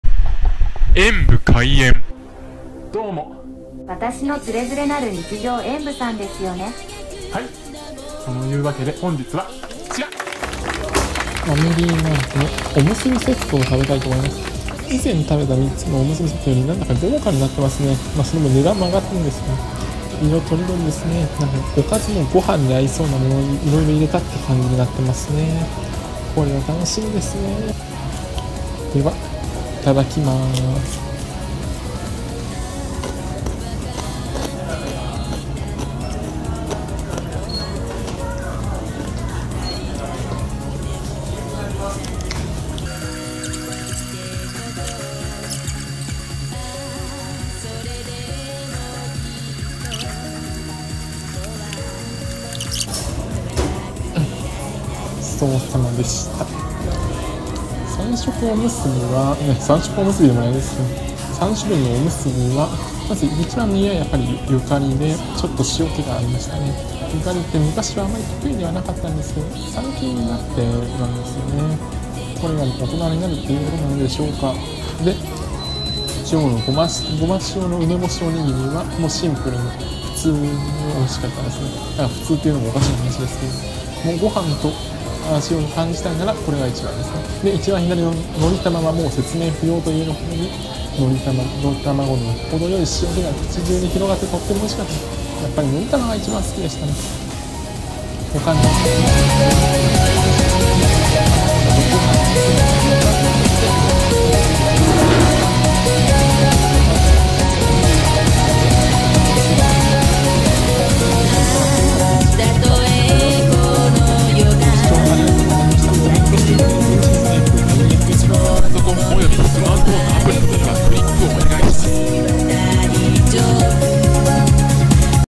ファミリーマートの三色おむすびセットがパワーアップしたので改めて食べてみた
ファミリーマートの三色おむすびセットがいつの間にかパワーアップしていましたので食べてみました。改めて食べると又違いますね～
贈り物は此方まで↓
〒153-0042